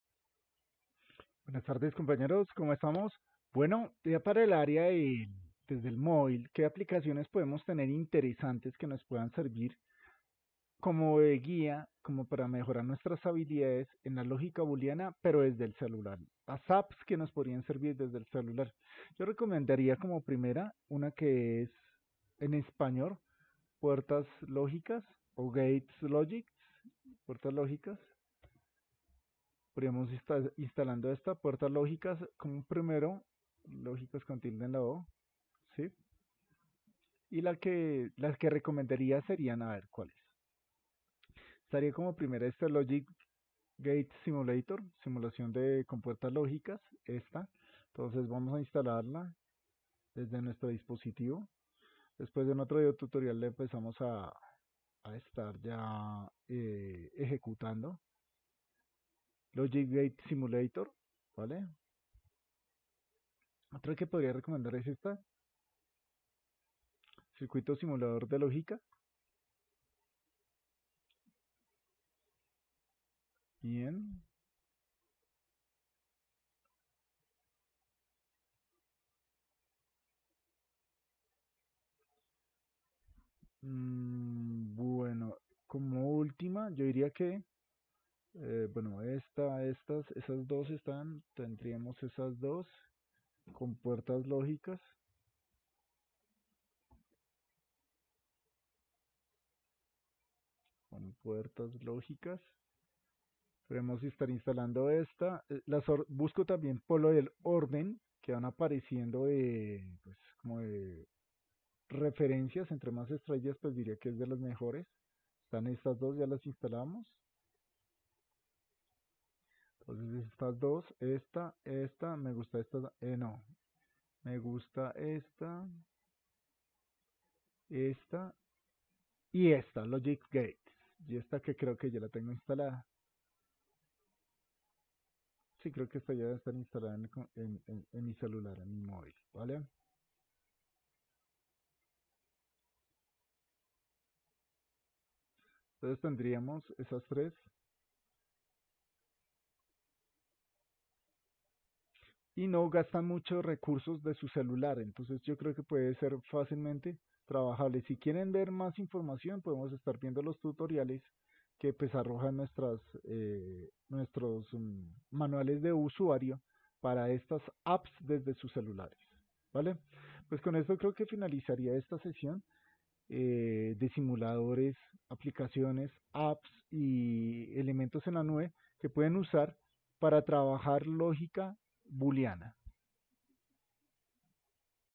APP´s en Android - Simuladores Emuladores Electrónica
App´s desde Android para generar conocimiento y habilidad de Compuertas lógicas desdé su móvil con Android